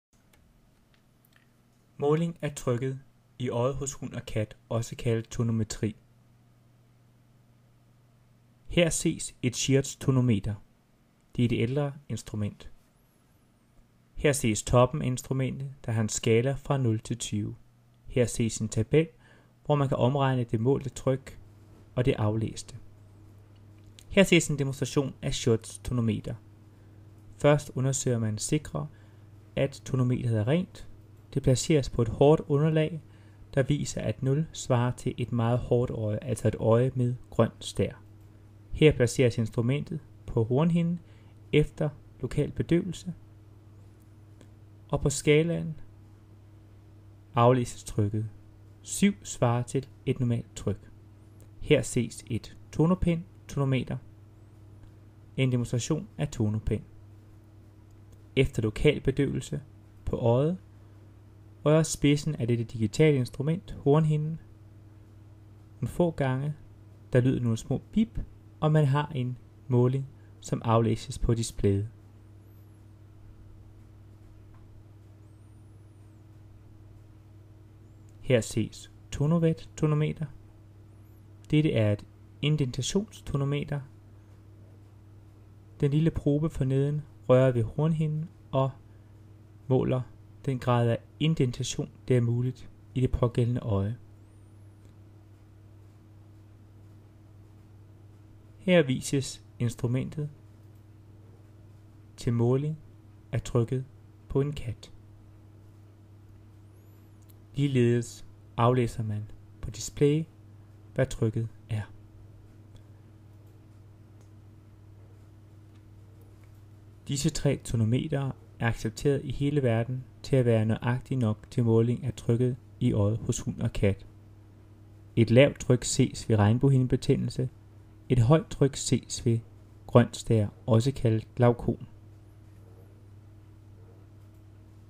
Tonometri - Schiotz - Tonopen - Tonovet - Tonometer demonstration på dyr
Måling af trykket i øjet hos hund og kat (tonometri). Trykket måles efter lokal bedøvelse er givet i form af øjendråber. Alle tre tonometre er anset for nøjagtige til bestemmelse af trykket (intraokulære tryk) hos hund og kat. Demonstration fra - erfaring i alle øjensygdomme hos dyr.